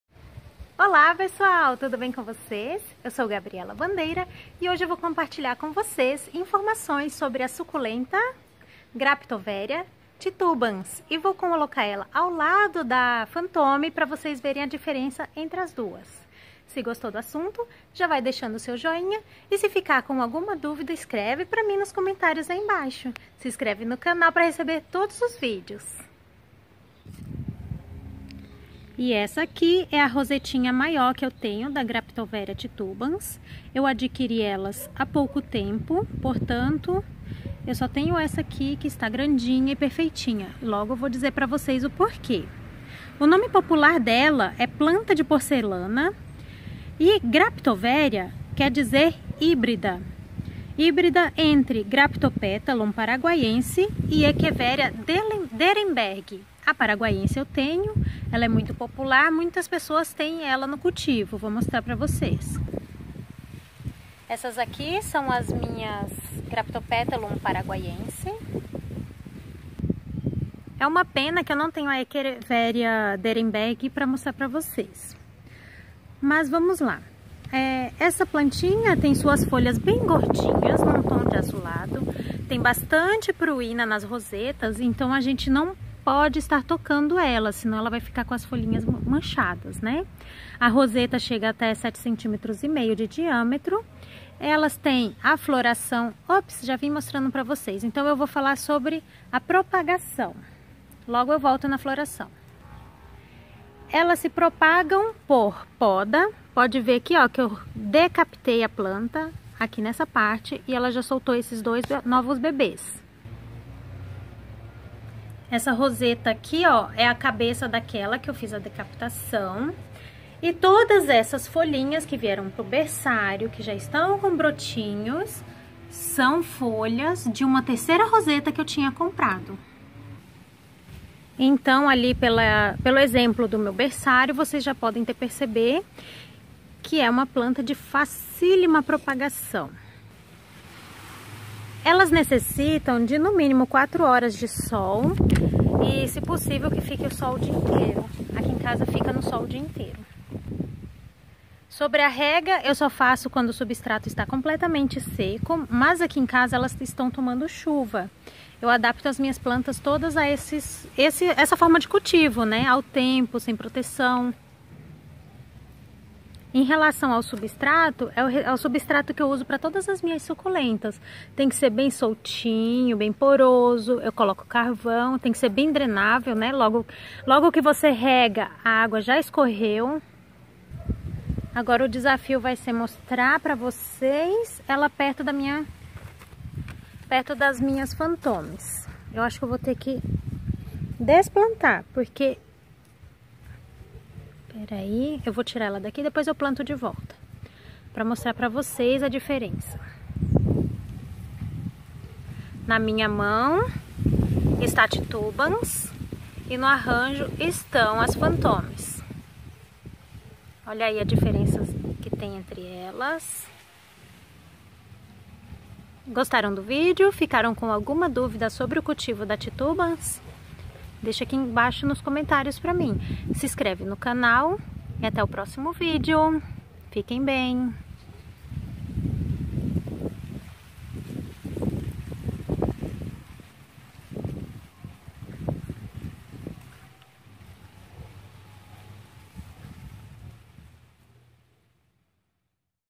Graptoveria titubans dicas de cultivo e comparação com Fantôme - Jardim da Gabi
No vídeo de hoje compartilhei com vocês algumas informações sobre a Graptoveria titubans .  Falei sobre como cultivar essa linda suculenta e como fazer a propagação. e ainda a coloquei pertinho da graptoveria fantôme pra vocês  observarem as diferenças  entre elas.